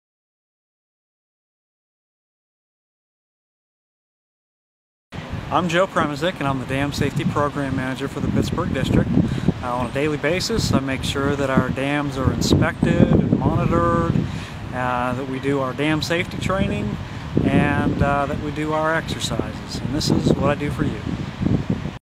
What we do
Pittsburgh District manages 16 flood-risk reduction facilities including Stonewall Jackson Dam seen in the background of this video. In 2017, throughout the district, our reservoirs, helped to prevent more than $530,000,000 in flood damages. Joe Promozic, Dam Safety Program Manager, explains what his roles is in this mission.

This video was originally published on our Facebook page on May 21, 2018.